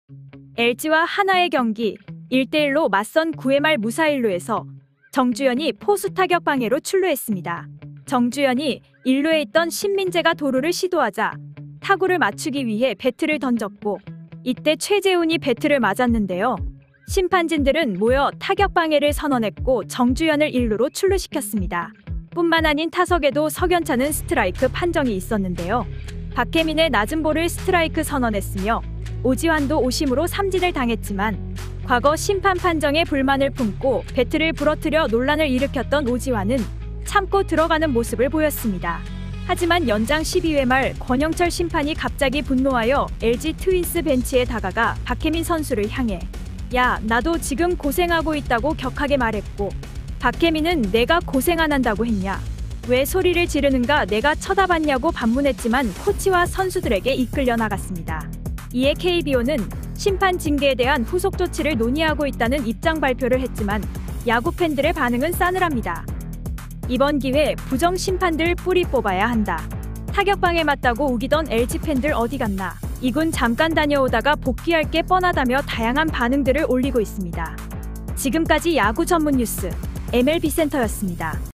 배트 던졌지만 '타격 방해' 선언.. 심판이 분노하며 박해민에게 고함 논란
엘지 한화 타격 방해 오심 박해민 권영철 심판 오심 KBO 징계